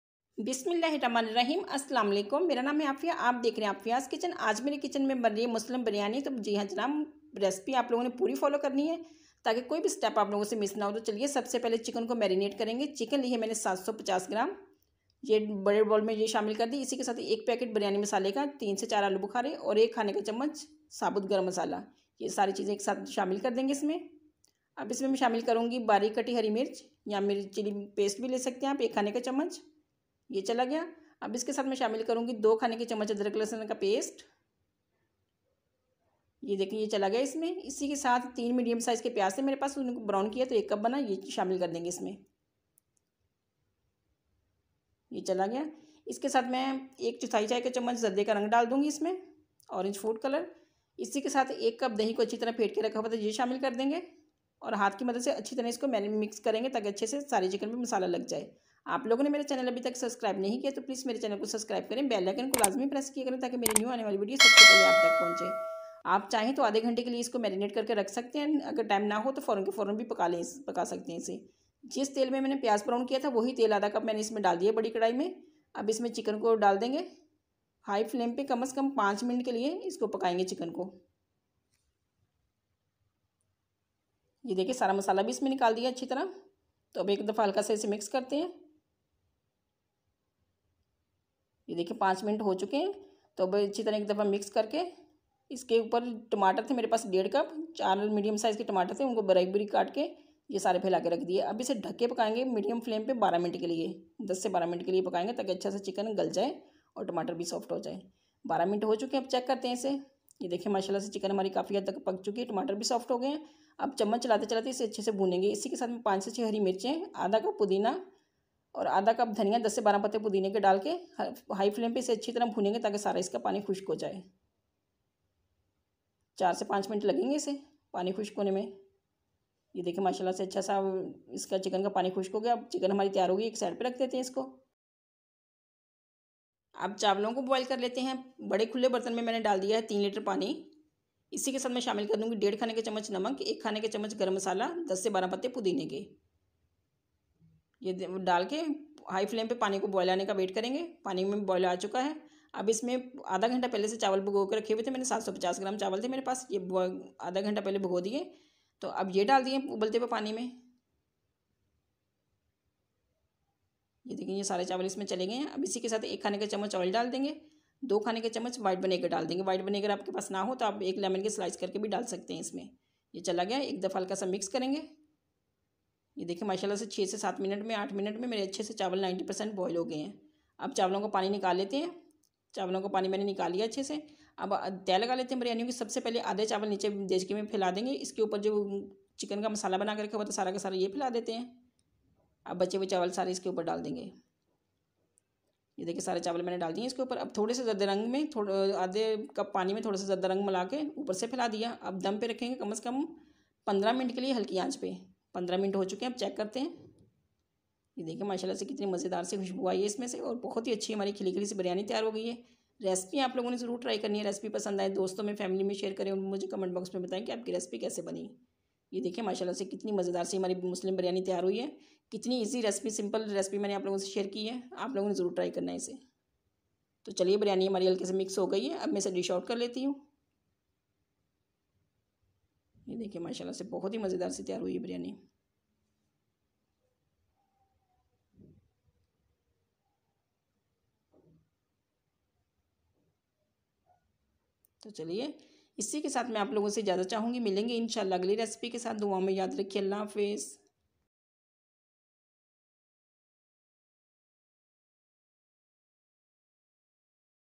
Muslim biryani complete recipe in Urdu / Hindi by Afia's kitchen
how to make Muslim biryani at home.
step by step complete recipe.